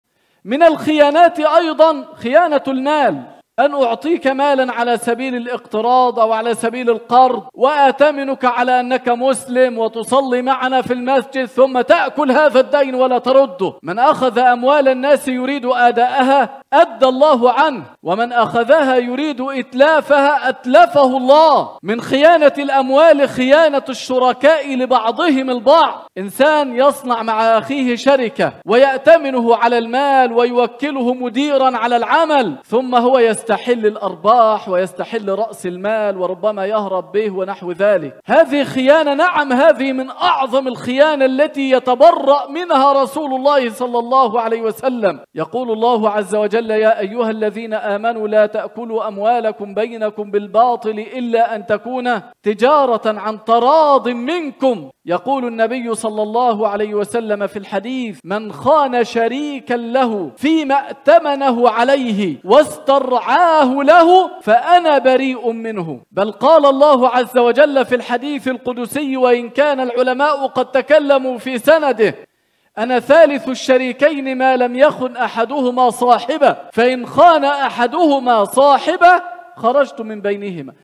خيانة إذا فعلتها يتبرأ منك رسول الله
يسعدنا تواصلكم معنا من خلال الرابط التالي

البرنامج الدعوي من انتاج شركة صوت الدنيا  للانتاج الفني والتسويق الالكتروني
تقديم الدكتور " السيد الشرقاوي " المدرس المساعد بجامعة الازهر الشريف
ــــــــــــــــــــــــــــــــــــــــــــ
ــــــــــــــــــــــــــــــــــــــــــــــ